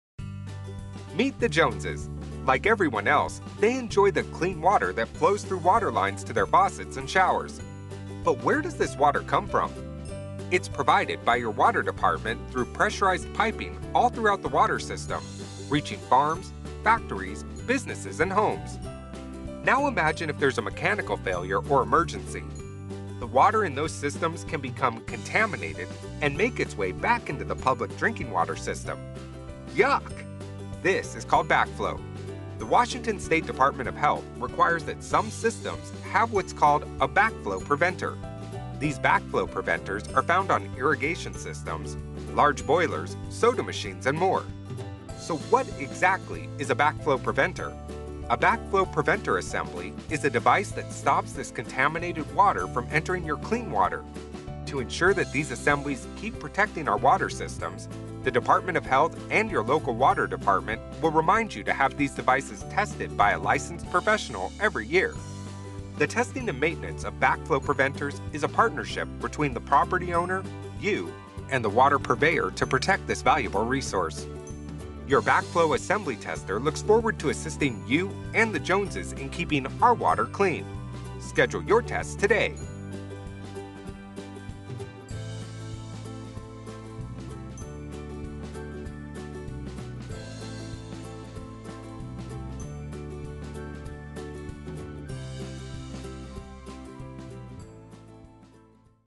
What is backflow?
Meet the Jones while they learn about what a Backflow Assembly Device is and what it means for them.